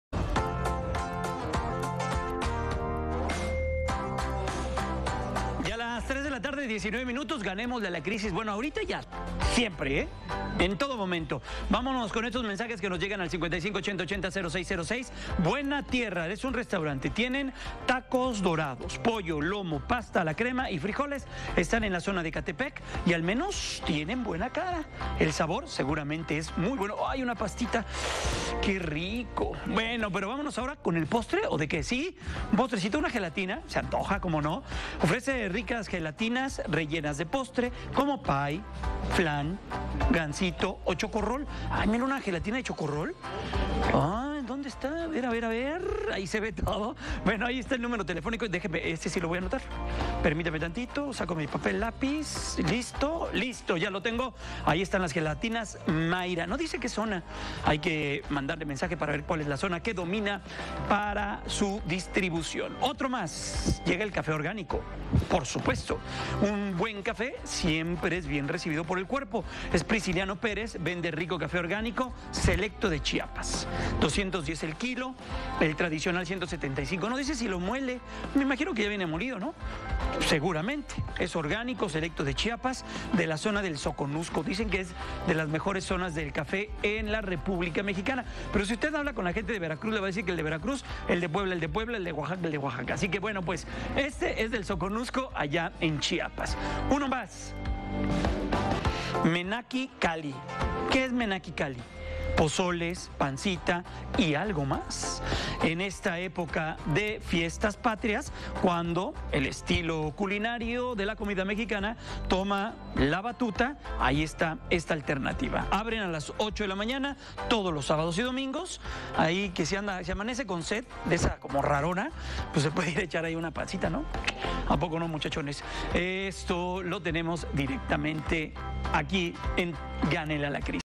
Gánale a la Crisis: Gelatinas, Café Orgánico y Comida Mexicana
- Restaurante “Buena Tierra”
- Gelatinas Mayra
- Café Orgánico
- Menaki Kali (Pozole, Pancita)